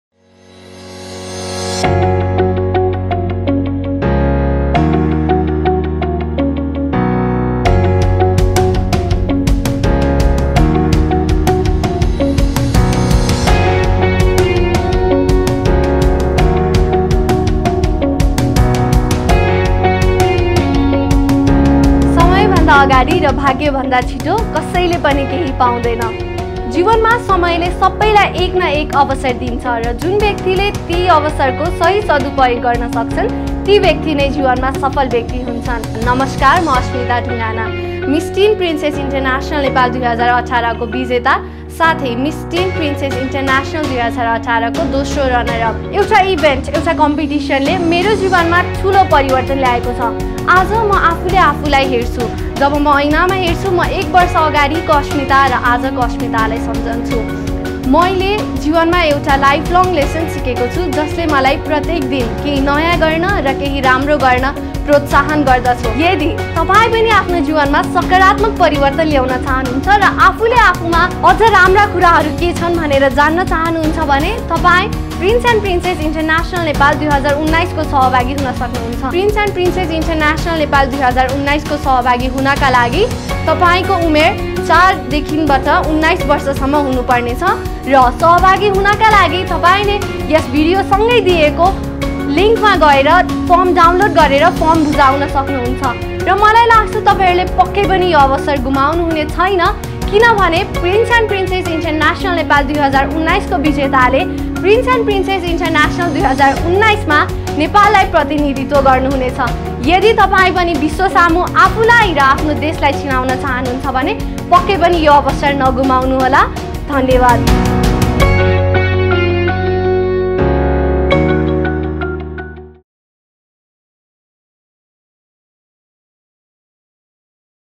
PPIN 2019 ANNOUNCEMENT_Miss Teen International 2nd Runner Up Asmita Dhungana
ANNOUNCEMENT !! ⓐⓝⓝⓞⓤⓝⓒⓔⓜⓔⓝⓣ !!
"PRINCE & PRINCESS INTERNATIONAL NEPAL 2019"
HURRY UP Boys and Girls (4-19 years) for NATIONAL & INTERNATIONAL recognition..
Those Who Have Not Submitted The Form Please Send Us Soon !!!

Mini Miss & Mr. Prince & Princess International Nepal
Age : 4-7 yr. boys and girls.
Little Miss & Jr. Prince & Princess International Nepal
Age : 8-11 yr. boys and girls.
Pre-Teen Miss & Mr. Prince & Princess International Nepal
Age : 12-14 yr. boys and girls.
Miss & Mr. Teen Prince & Princess International Nepal
Age : 15-19 yr. boys and girls.
Group of Event Entertainers
Pulchowk, Damkal Mode, Lalitpur